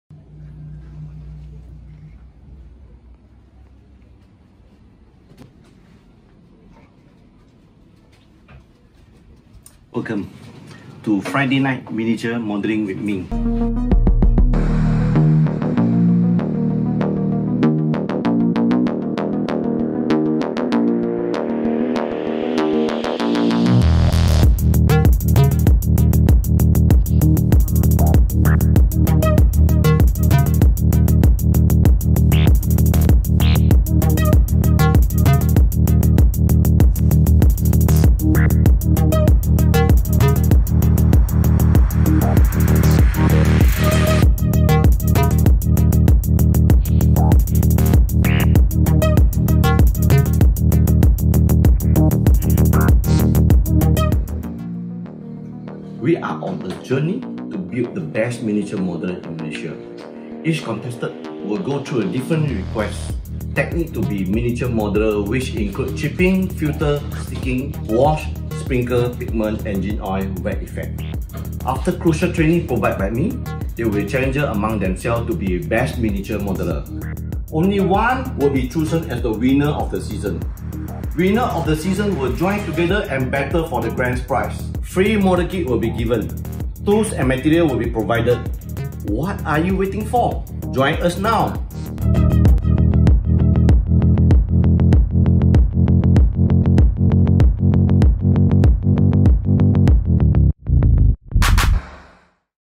UPCOMING FRIDAY NIGHT MINIATURE MODELLING with MING TRAILER
The first Reality Miniature Modelling Show screening at every Friday 8.00pm, every week starting from 15th January 2021, a total of Four Season, 4 Episodes every season.

Each Season of the show, modelers gets free kit, materials, paints and free tutorial. At the end of each season, only one winner will be selected to compete in the grand finale.

Only 3 modelers will be allowed in a show as to observed the Standard Operation Procedure, no visitors/spectators will be allowed to view the show during live recording.

On the Grand Finale at the end of the season, Four winning contestants from season 1,2,3 & 4 will compete against each other to be declared the winner of this challenge.

Don’t miss this exciting Prelude episode of the Friday Night Miniature Modelling at 15th January, Friday, 2021.

Interested to sign up as contestant?